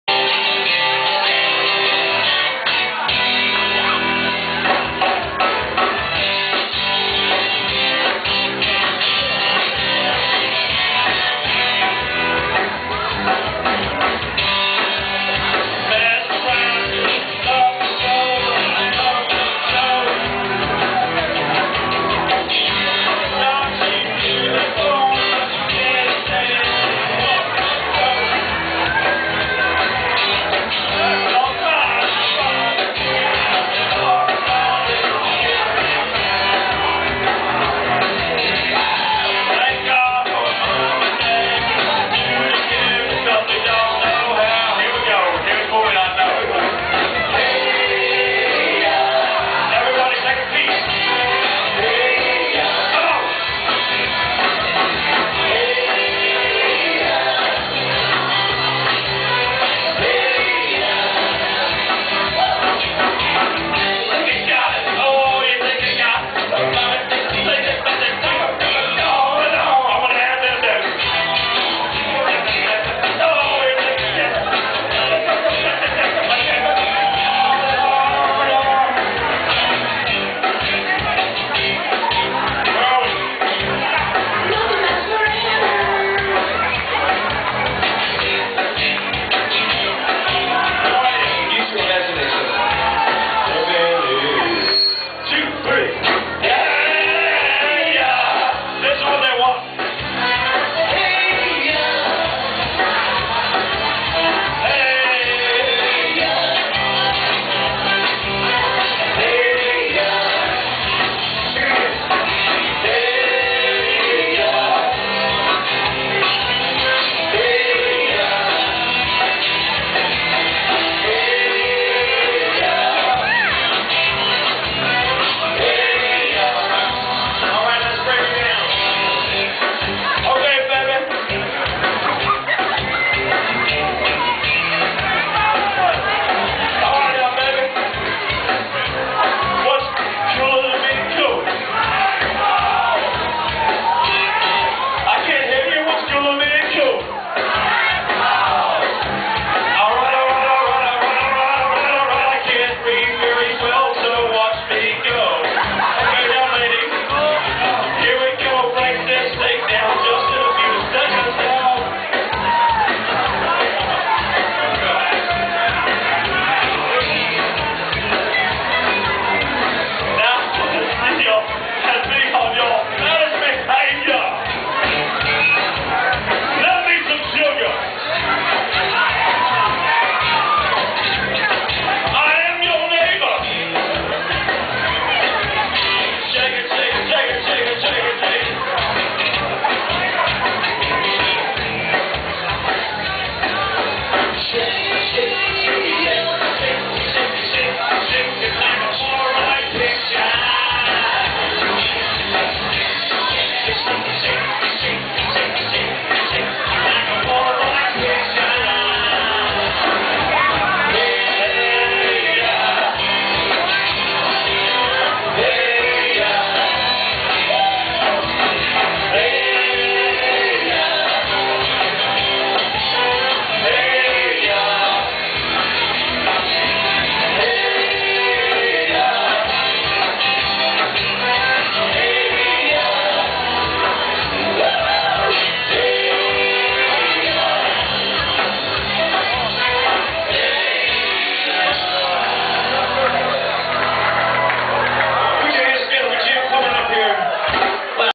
1-30-09 ROCKSTAR FRIDAYS @ STUMPS
1-30-09  ROCKSTAR KARAOKE  STUMPS SUPPER CLUB  615 CHANNELSIDE DRIVE  TAMPA, FL.